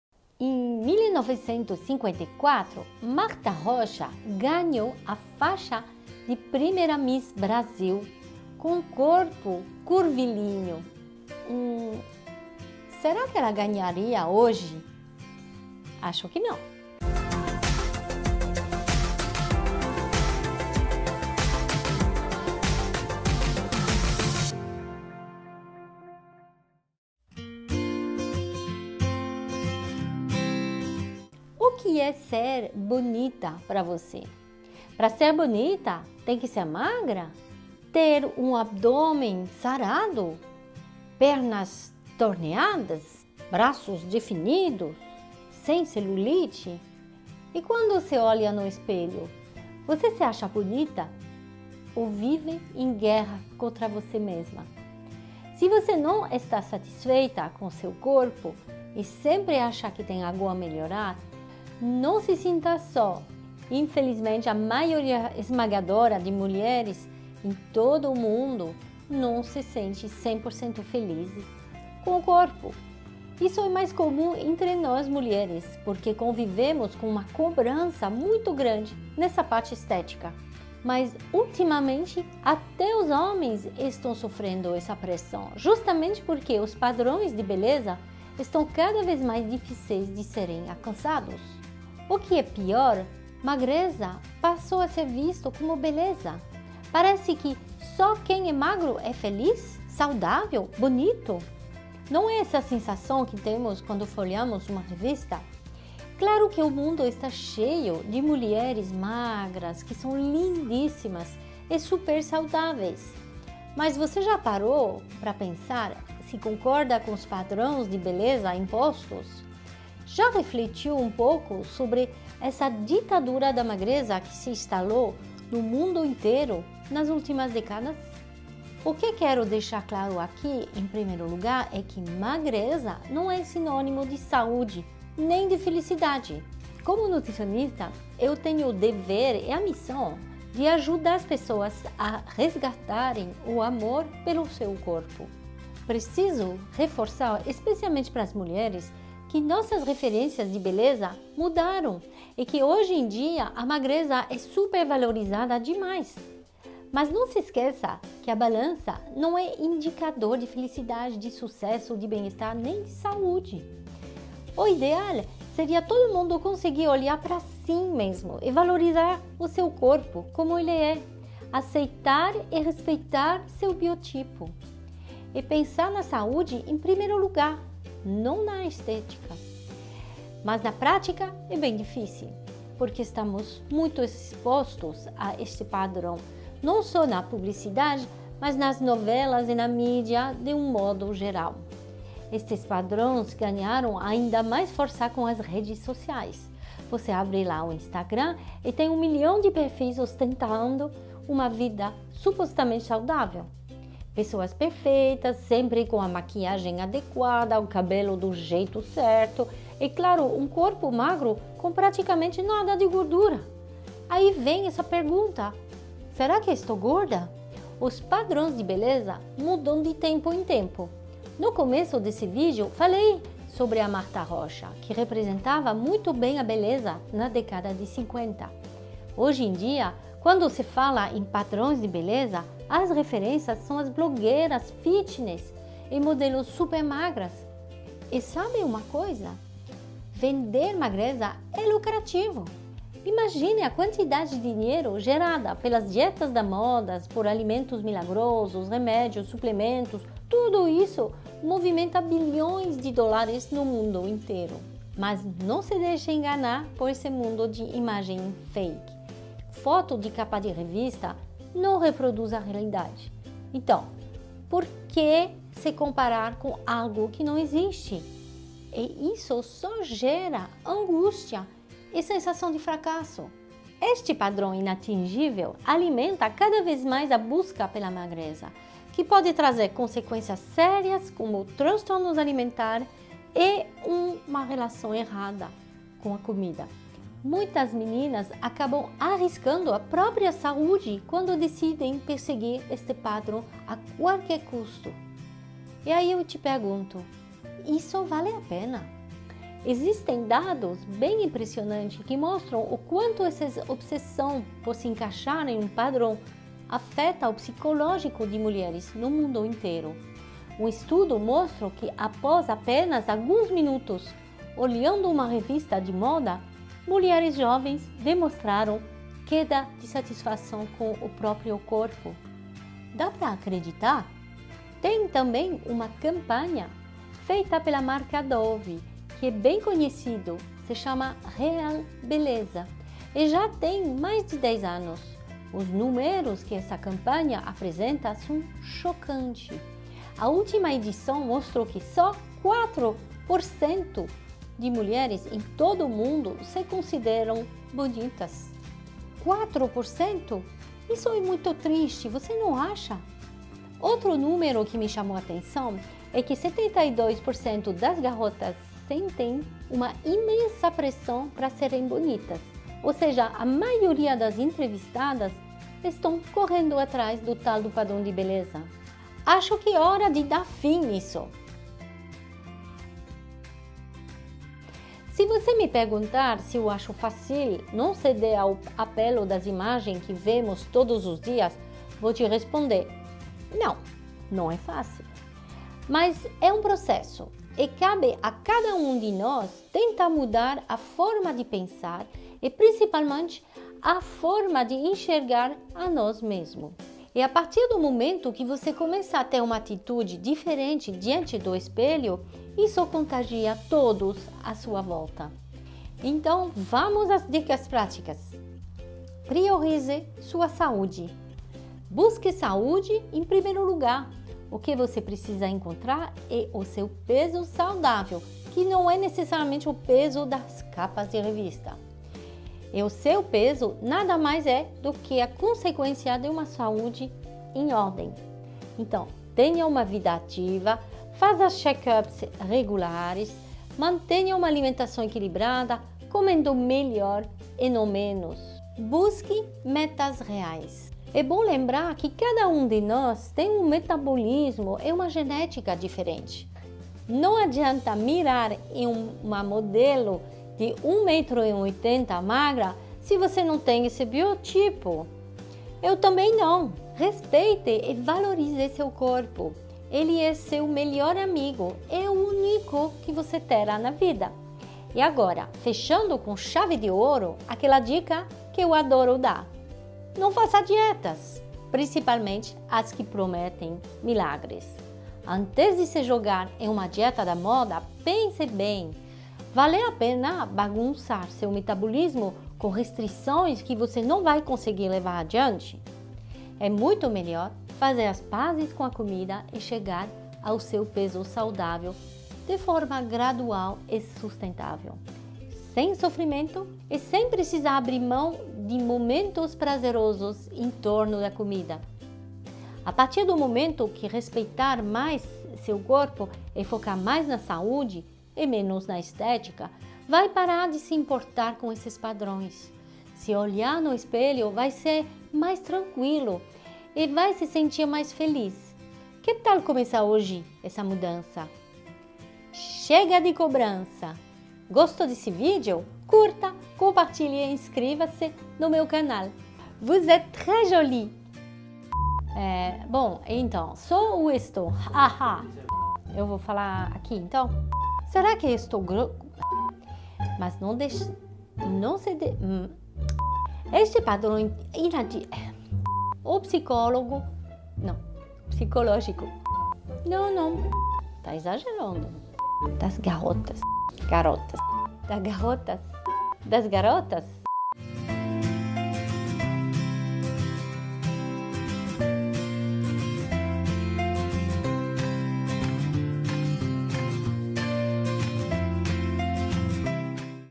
Padrões de Beleza
O que é ser bonita? É ser magra, não ter celulite?
Você se acha bonita ou vive em guerra com você mesma?
Infelizmente isso é muito comum nas mulheres, mas hoje em dia, com a pressão estética, homens também sofrem com isso.

Hoje, a magreza virou o padrão de beleza. Mas, há beleza e saúde em todos os corpos!
Devemos lembrar que magreza não é sinônimo de saúde. E que balança não é indicador de sucesso, felicidade e saúde.

Que tal tentar olhar para si mesma? E valorizar seu corpo, como ele é. Aceitar e respeitar o seu biotipo. :)
Pensar na saúde em primeiro lugar e não na estética! :-)
É um processo longo e até difícil, mas vale a pena!

Afinal, padrões de beleza mudam de tempo em tempo. E estar insatisfeita com o corpo, gera lucro para muitos. Já pensou nisso? ;-)

Transforme sua relação com a comida. Vamos fazer as pazes com a comida? -Saia do efeito sanfona e entre no efeito Sophie. Coma melhor e não menos.